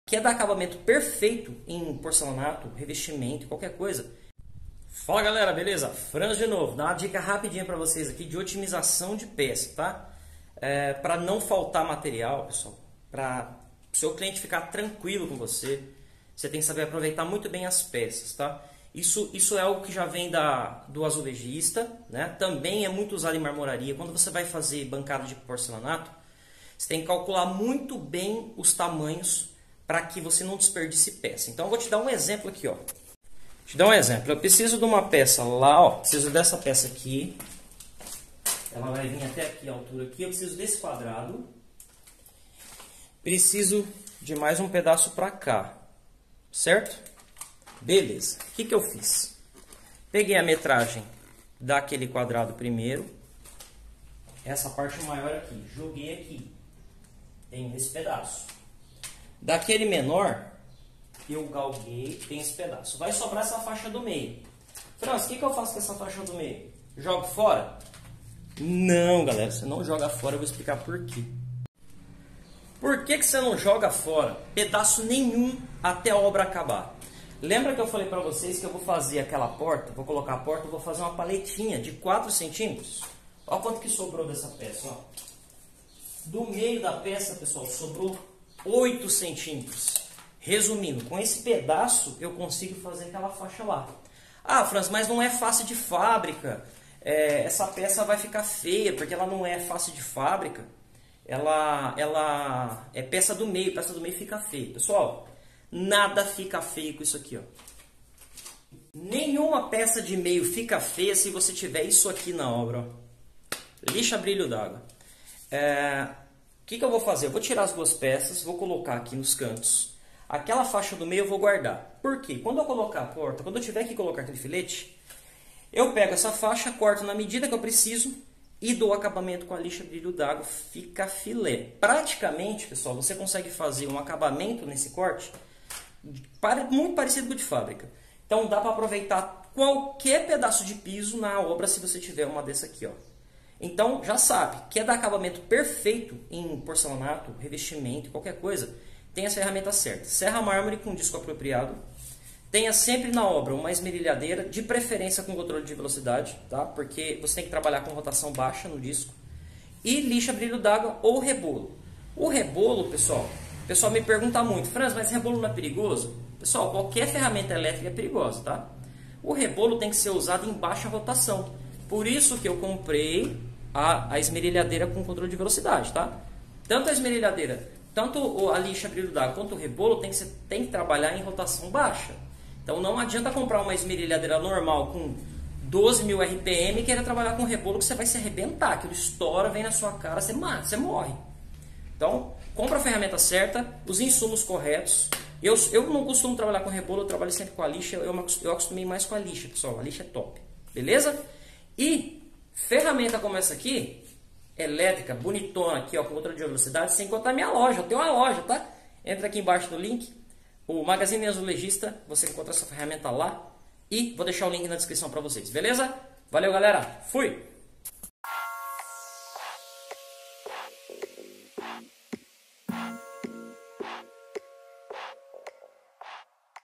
Posso usar peça de meio no assentamento? Com essa ferramenta pode.
Como fazer acabamento perfeito no piso.A melhor ferramanta é esta

Neste vídeo vou te mostrar a melhor ferramanta para fazer acabamento perfeito no piso e porcelanato.
A esmerilhadeira com contole de velocidade para usar rebolo ou lixa você encontra com ótimo preço na nosso loja: